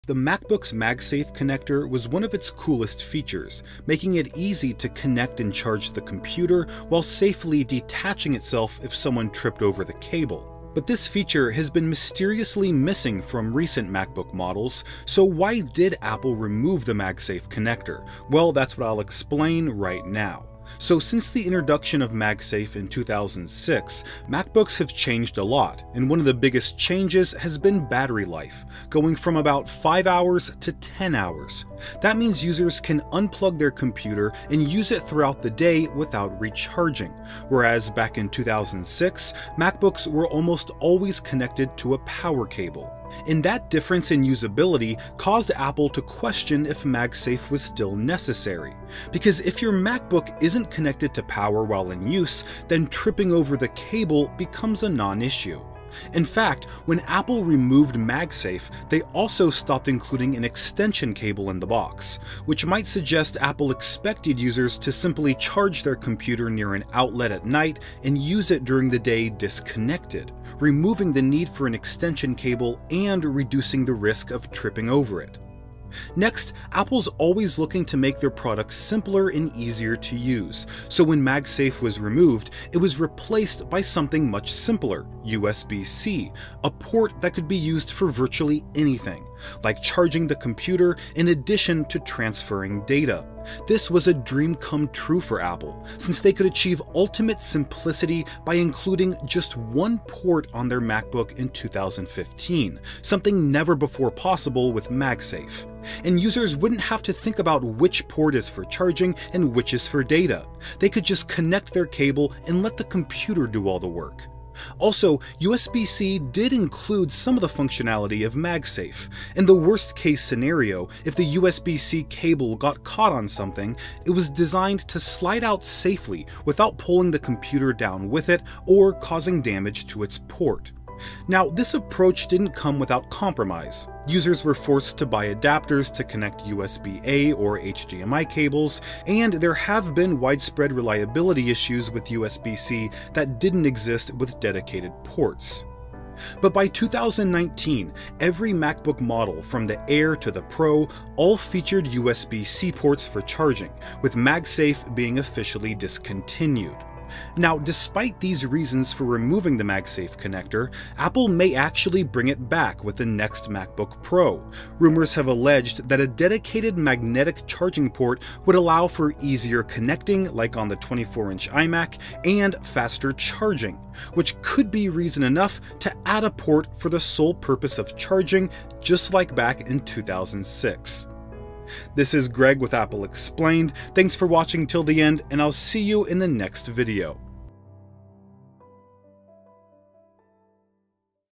Why The MacBook's MagSafe Connector Was Removed
The MacBook’s MagSafe connector was one of its coolest features. Making it easy to connect and charge the computer, while safely detaching itself if someone tripped over the cable. But this feature has been mysteriously missing from Apple's recent MacBook models. So why did Apple remove the MagSafe connector? Well that’s what I’ll explain in this video.